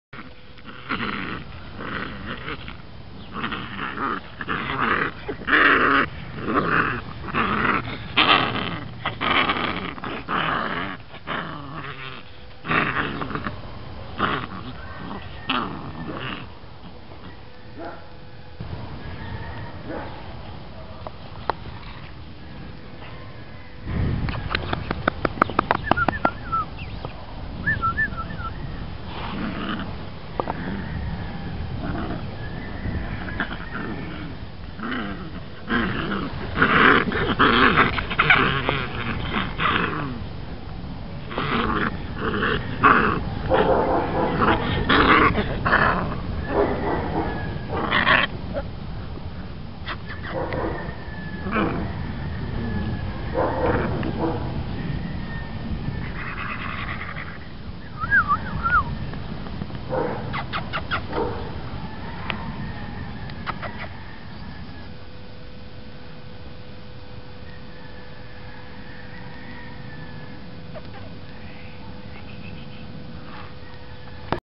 seguridad canina centinel k9
esta es una cachorra de 5 semanas de nacida de max con una hembra de nombre vallira, esta camada es la primera cruza de max, caracter y temperamento garantizado.